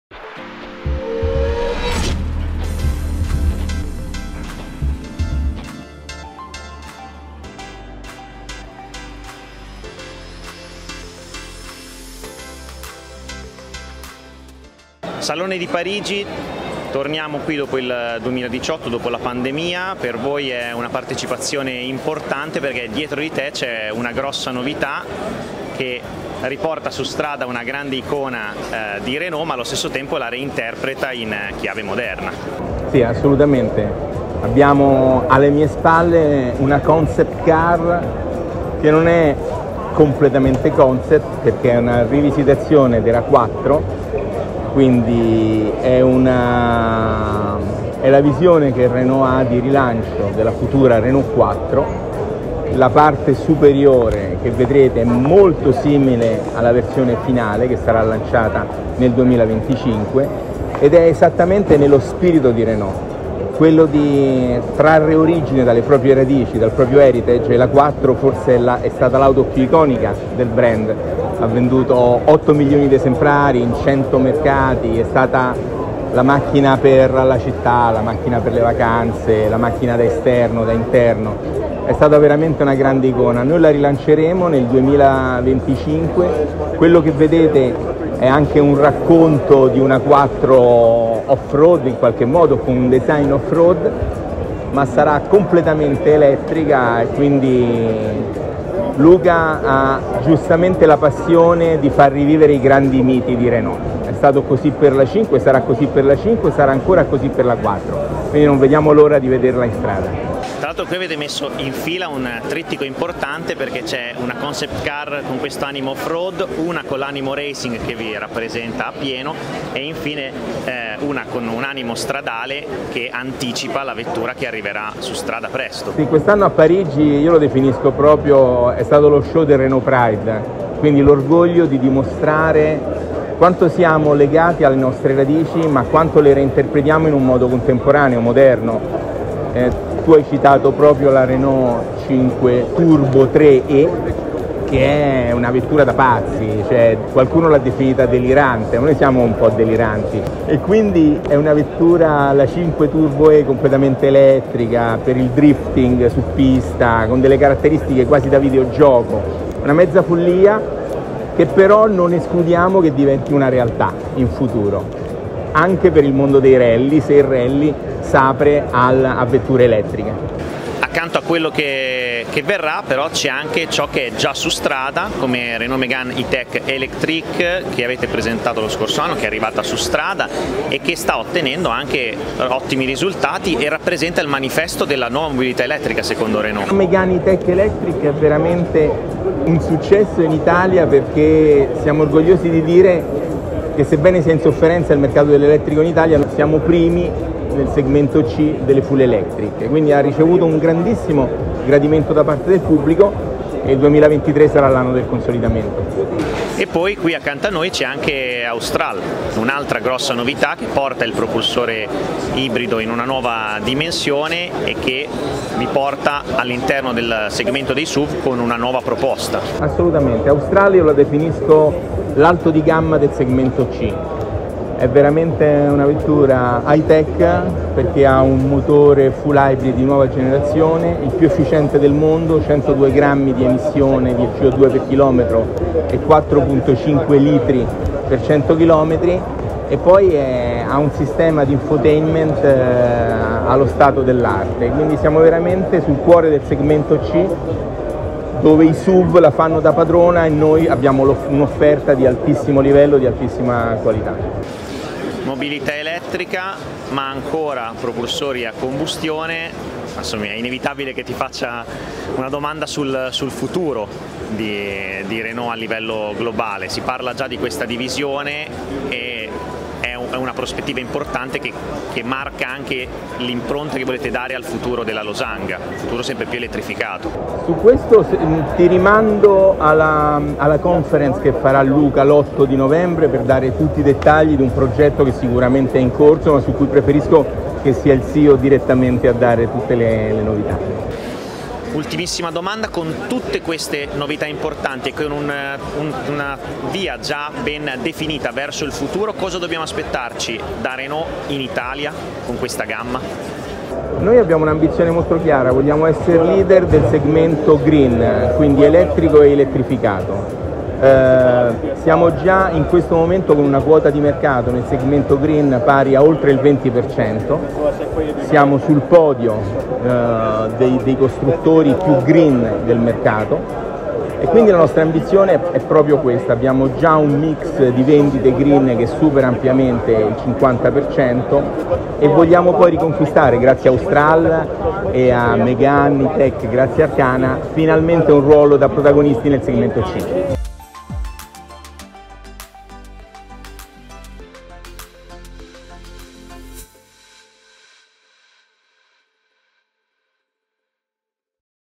Salone Parigi 2022 | Renault, intervista a Raffaele Fusilli
L'amministratore delegato di renault Italia Raffale Fusilli in esclusiva ai microfoni di FormulaPassion durante il parismotorshow per raccontarci delle novità della Losanga durante il Salone dell'Auto di Parigi ma anche per guardare ai programmi per l'Italia e all'evoluzione della gamma con l'arrivo di Megane E-Tech Electric e di Austral.